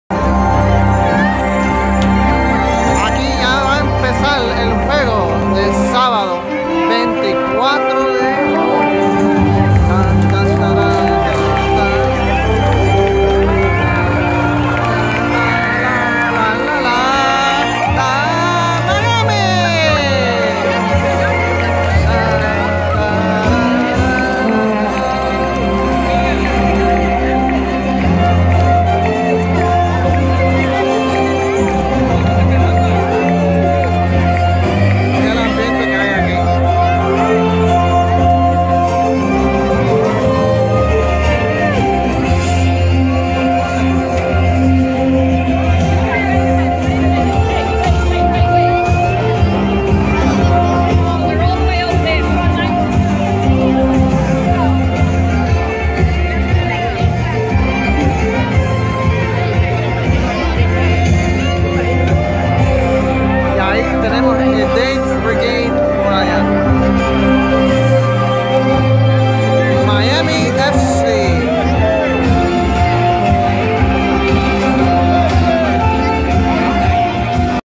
Miami FC saliendo con la música inspiradora
Miami le ganó a Nueva York 2-1 el Sábado 24  Junio 2017